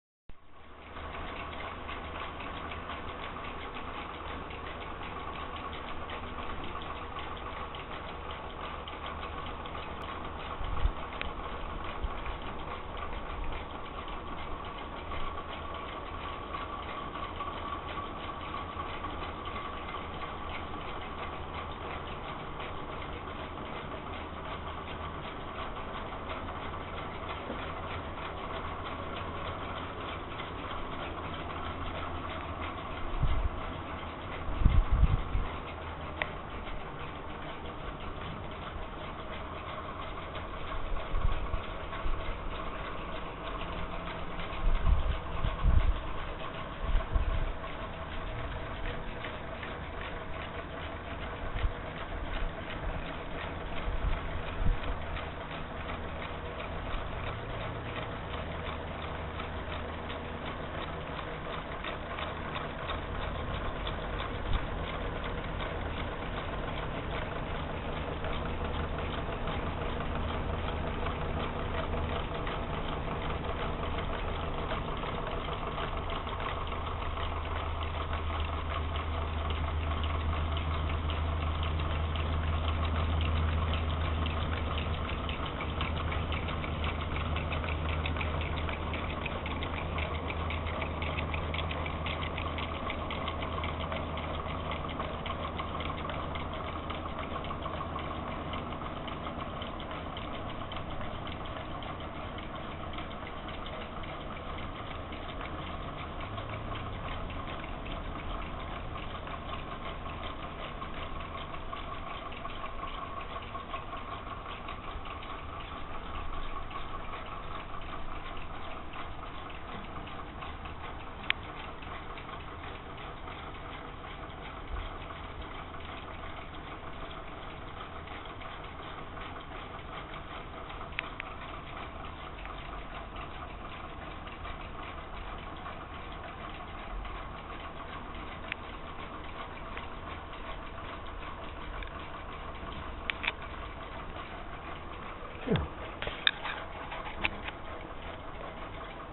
Whirligig Wars
The first W/gig is australian Aborigines paddleing a Bark Canoe driven for demonstration by a bathroom exhaust fan which also drives a celebration of Australia's Federation in 1901.